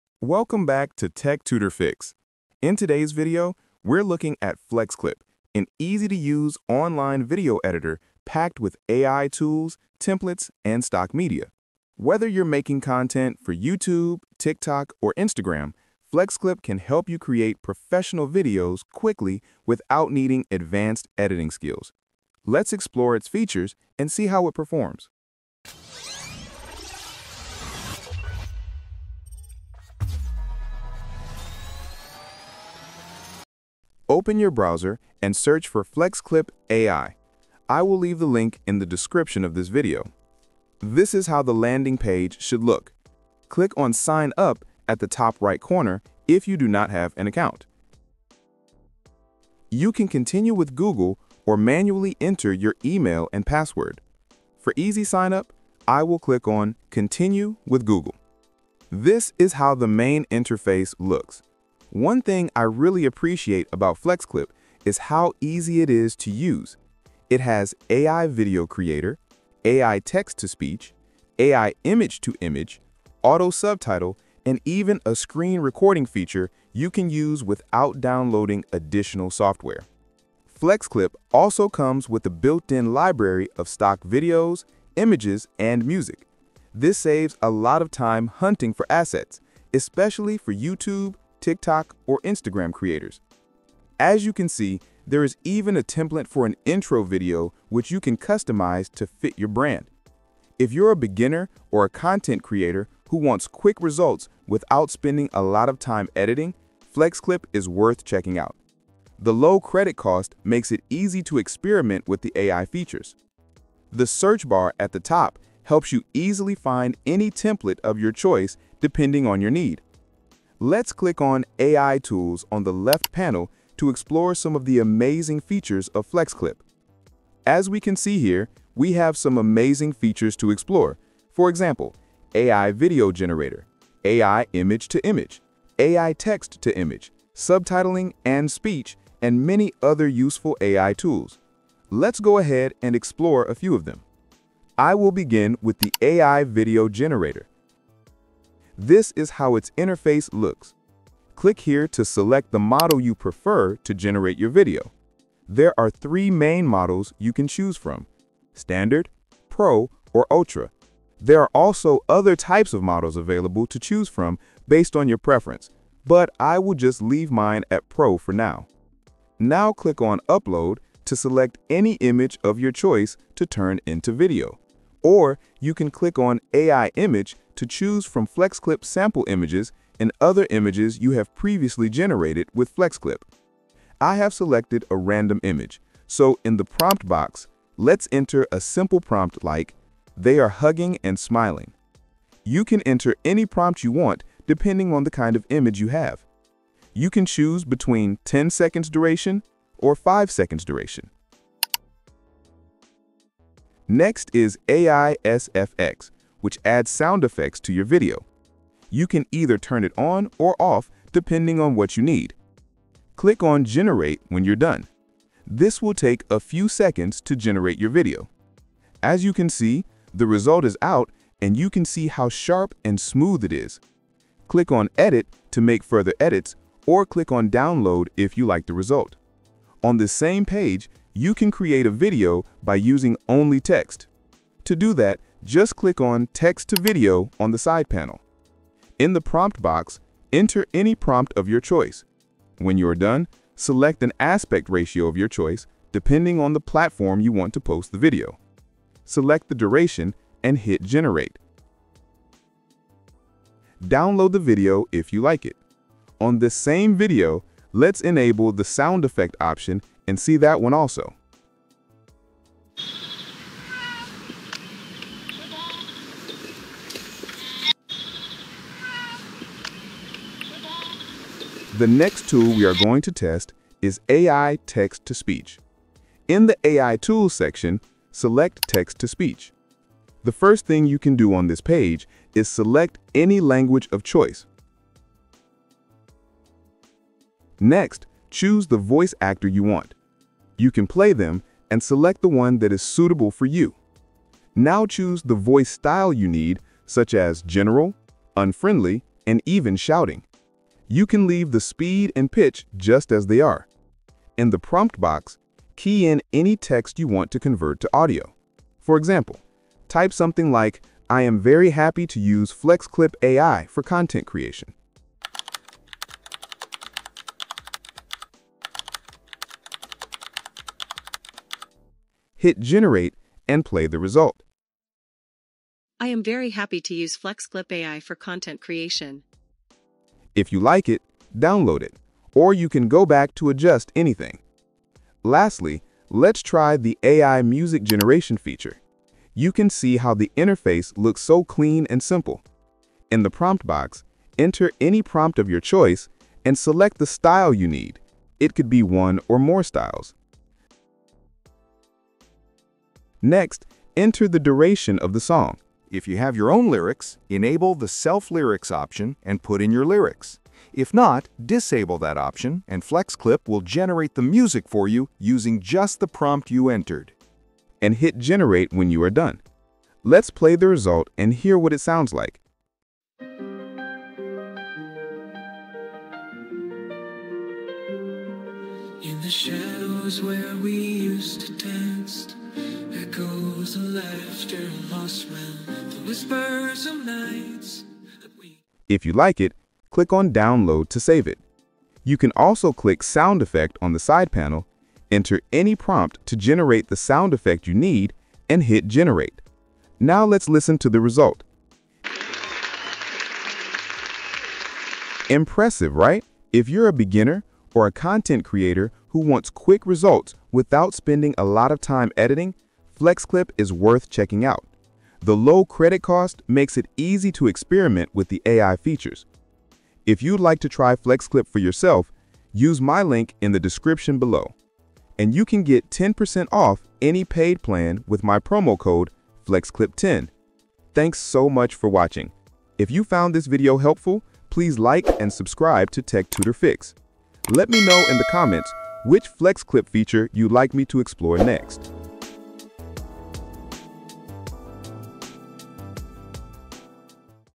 Create Pro Videos in Minutes – FlexClip AI Review
Looking for an easy way to create professional-looking videos without complicated software? In this video, I review FlexClip, an online AI-powered video editor packed with templates, stock footage, and powerful tools to help you create content fast.
FlexClip’s AI features make it easier than ever to:
✅ Turn text into video
✅ Use thousands of templates for YouTube, TikTok, Instagram
✅ Edit online without downloads
✅ Experiment with low-cost credits
Watch the video to see how FlexClip can help you create pro videos in minutes!